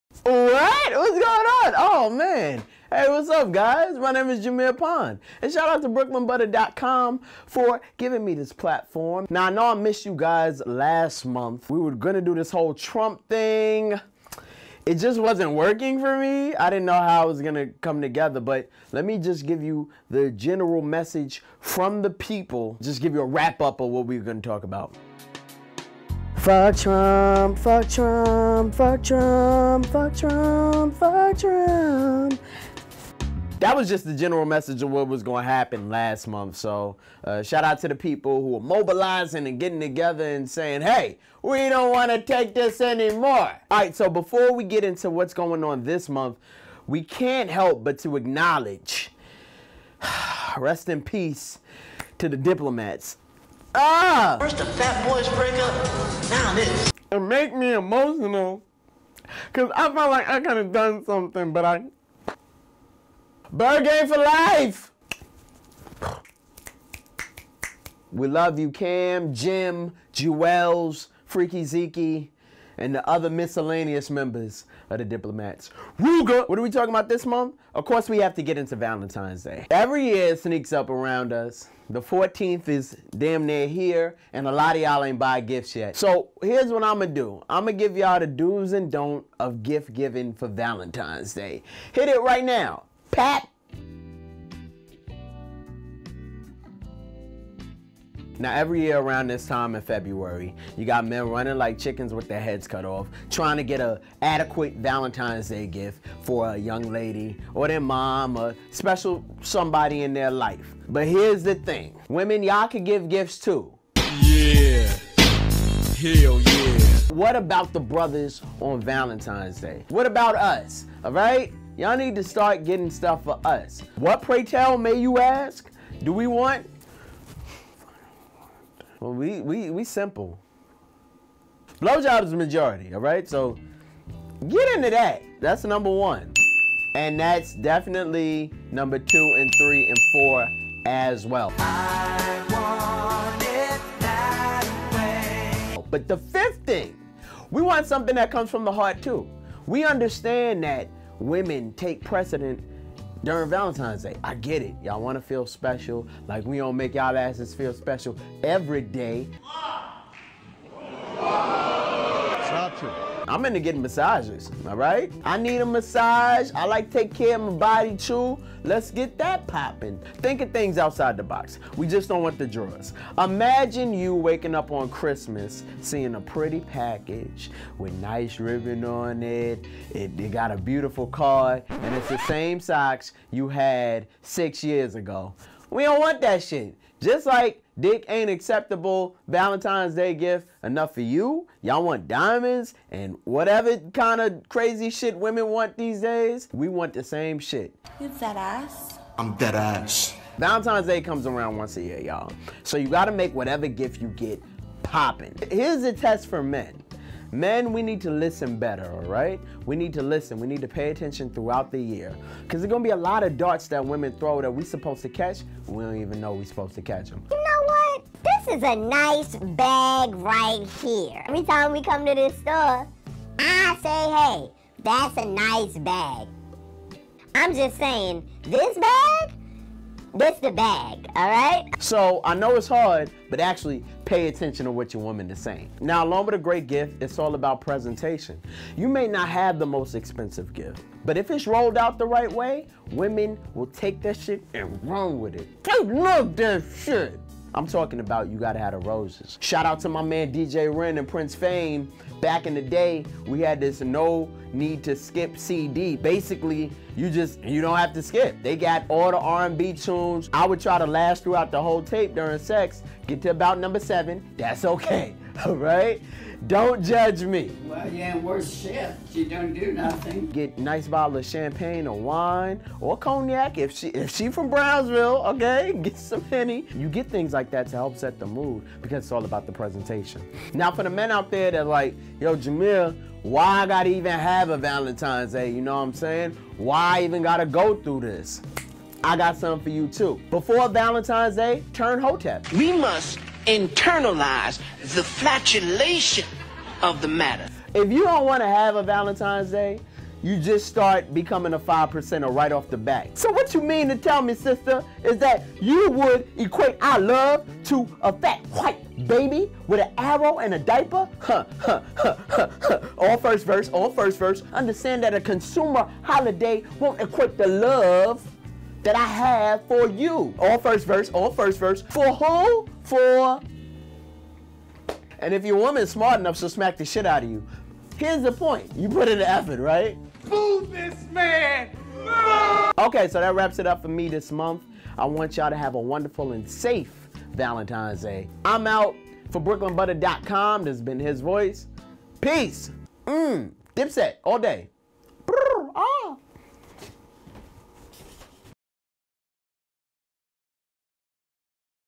HisVoice: Jameer Pond Tackles Valentine's Day
In this episode of His Voice, Jameer Pond talks all things Valentine's Day gift giving for both the fellas and the ladies.